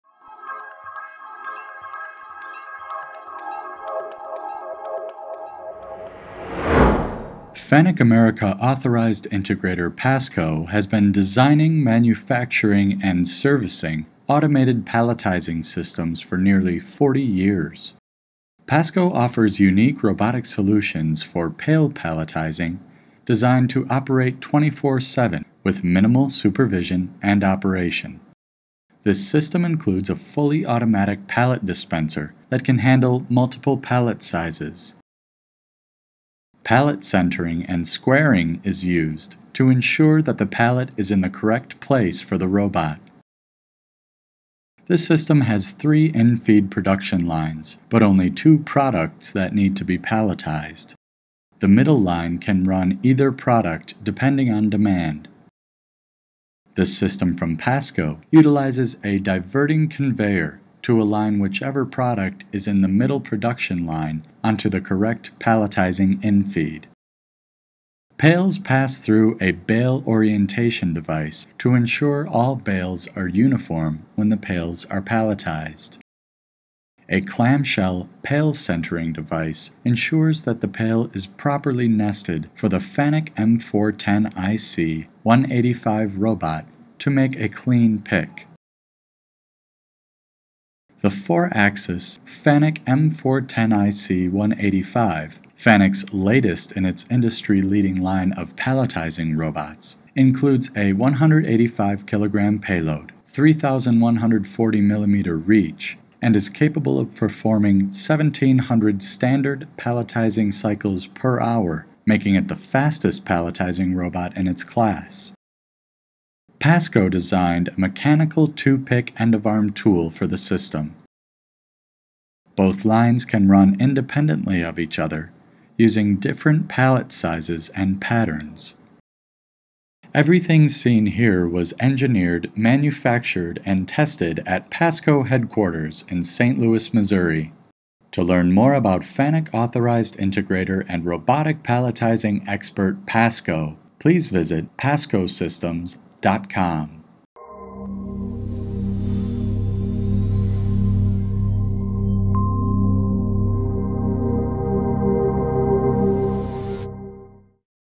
Robotic Pail Palletizing System with FANUC Palletizing Robot - PASCO
FANUC America Authorized Integrator PASCO has been designing, manufacturing and servicing automated palletizing systems for nearly 40 years.

PASCO offers unique robotic solutions for pail palletizing designed to operate 24/7 with minimal supervision and operation. This system includes a fully automatic pallet dispenser that can handle multiple pallet sizes. Pallet centering and squaring is used to ensure that the pallet is in the correct place for the robot.

This system has three infeed production lines but only two products that need to be palletized. The middle line can run either product, depending on demand. This system from PASCO utilizes a diverting conveyor to align whichever product is in the middle production line onto the correct palletizing infeed. Pails pass through a bail orientation device to ensure all bails are uniform when the pails are palletized. A clamshell pail-centering device ensures that the pail is properly nested for the FANUC M-410iC/185 robot to make a clean pick. The four-axis FANUC M-410iC/185 – FANUC’s latest in its industry leading line of palletizing robots – includes a 185 kg payload, 3,140 mm reach, and is capable of performing 1,700 standard palletizing cycles/hour, making it the fastest palletizing robot in its class.

PASCO designed a mechanical two-pick end of arm tool for the system. Both lines can run independently of each other, using different pallet sizes and patters. Everything seen here was engineered, manufactured and tested at PASCO headquarters in St. Louis, Missouri. To learn more about FANUC Authorized Integrator and robotic palletizing expert PASCO, please